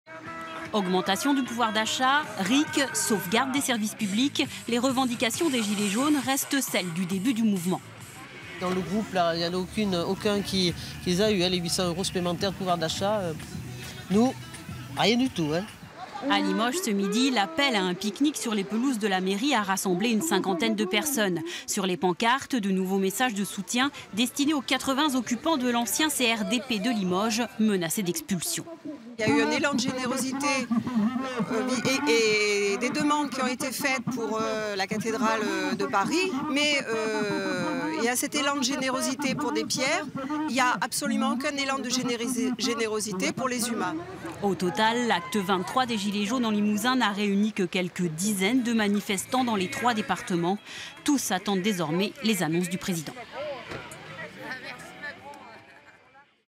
23eme acte gilets jaunes Limoges
Le weekend ensoleillé de Pâques n’a pas incité les manifestants à se mobiliser dans les 3 départements limousins.

Reportage Noëlle Vaille, Pascal Coussy, Sébastien Passelergue